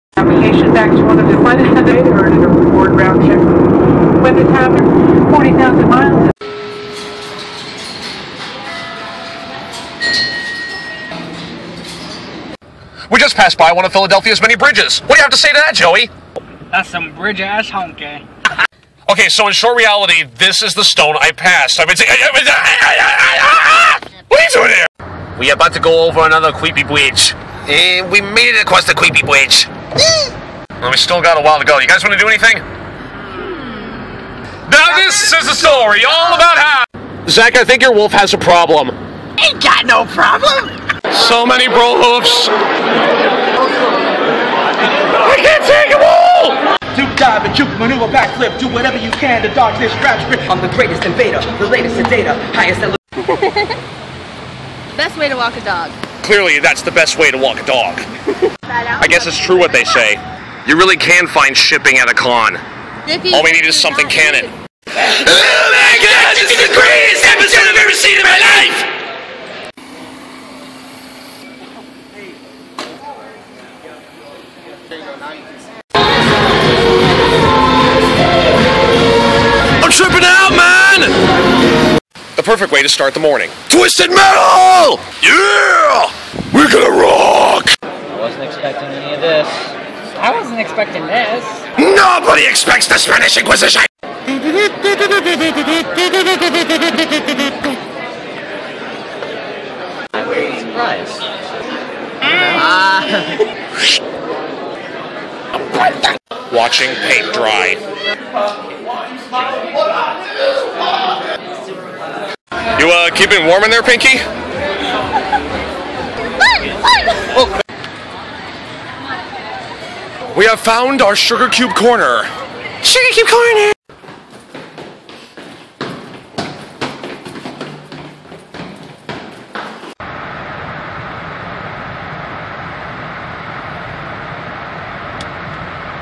BronyCon 2013 Vine Compilation
A simple compilation of all the vines I made while at BronyCon 2013 ^^